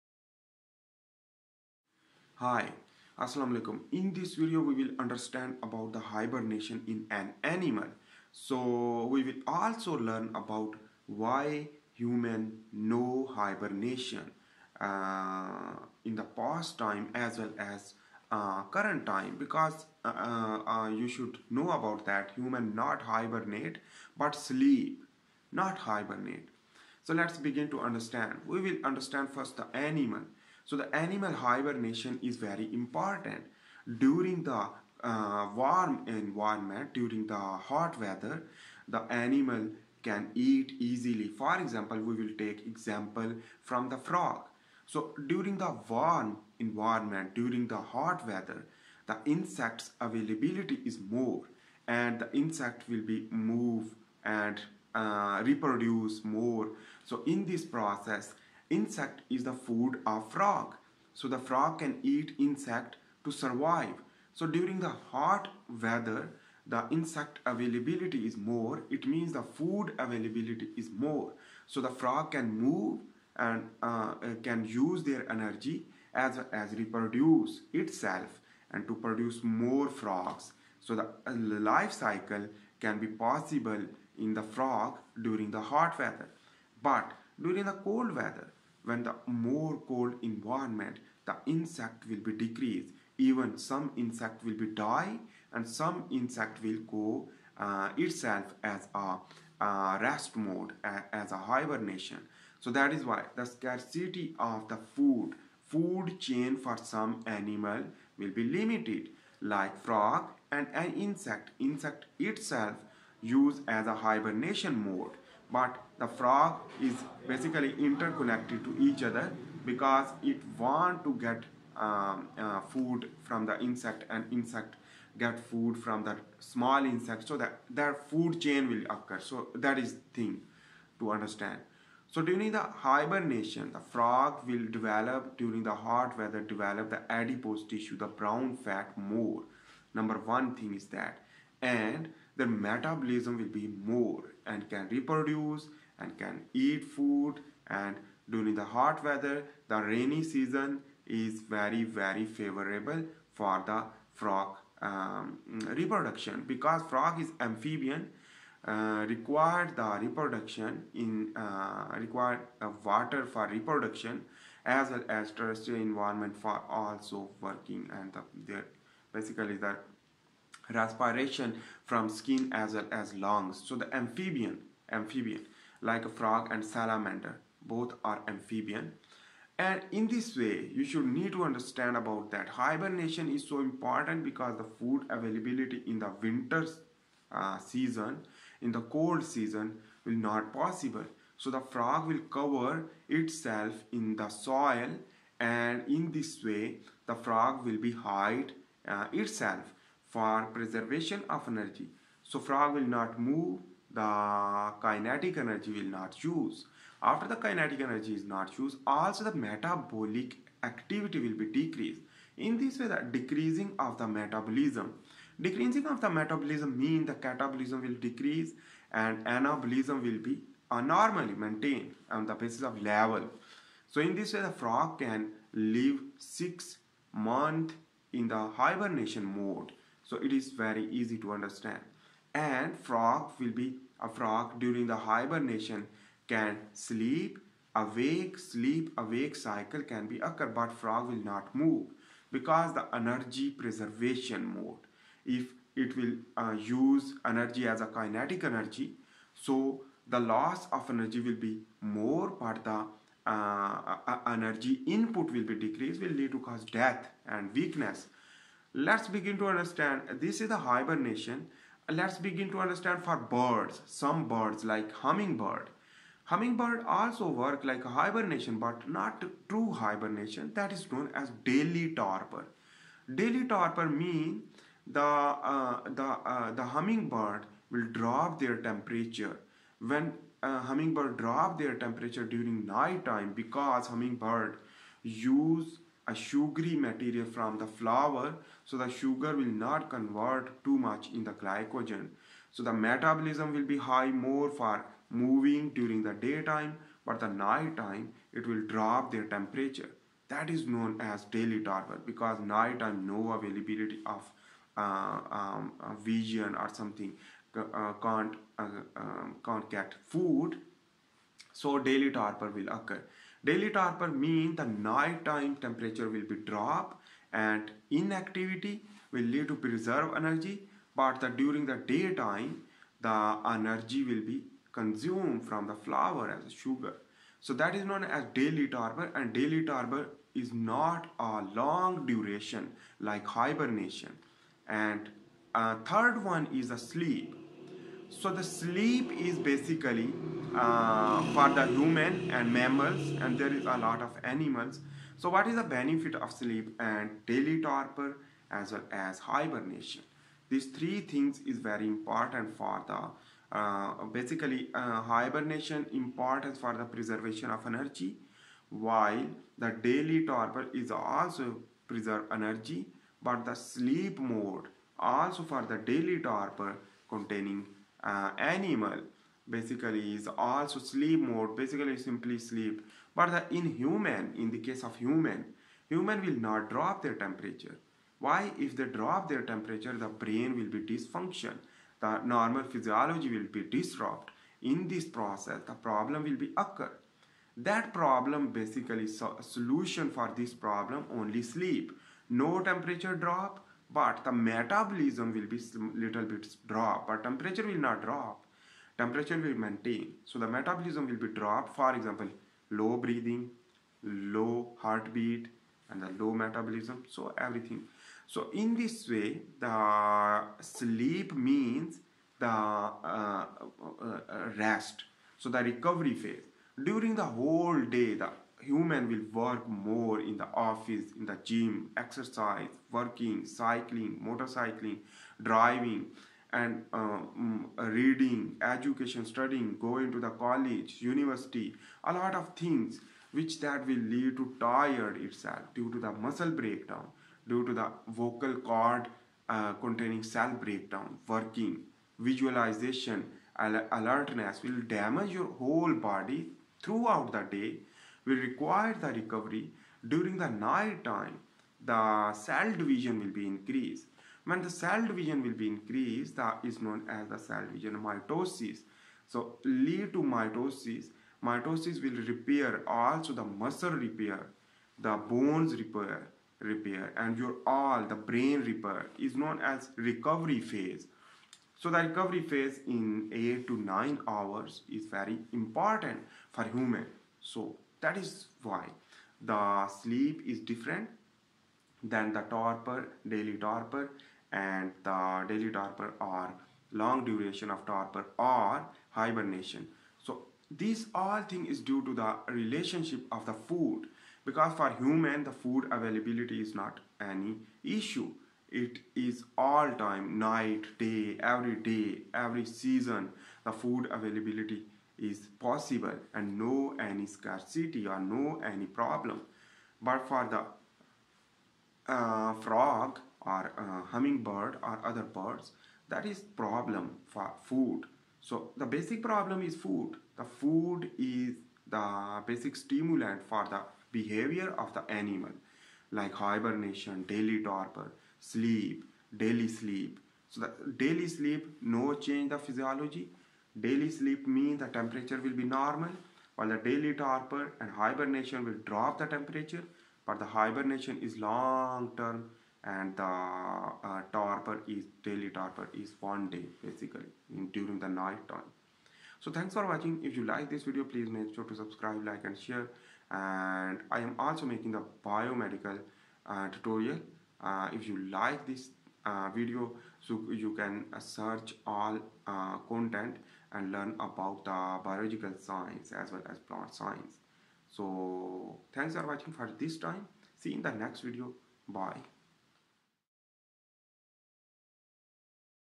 What Is Hibernation,Daily Torpor And Sleeping?
in this video i discussed about the What Is Hibernation,Daily Torpor And Sleeping?

click now to run videos

HIBERNATION :Hibernation is the process of inactivity of an animals in which 6 month inactivity due to lack of food available in cold environments like frogs
DAILY TORPOR :Daily torpor means the night time for humming birds no food available in case humming birds required sugar from flowers nectar.
in night time no any availablilty so the torpor will be possible so in this way humming birds relax to decrease the Metabolism,like hibernation but short duration
SLEEP:Sleeping is the unconsciously inactivity without drop temperature like hibernation so the temperature will be maintained and slow heart beat as well and decrease energy expenditure so the recovery will be possible.

I HOPE YOU GET THE CONCEPT
THANKS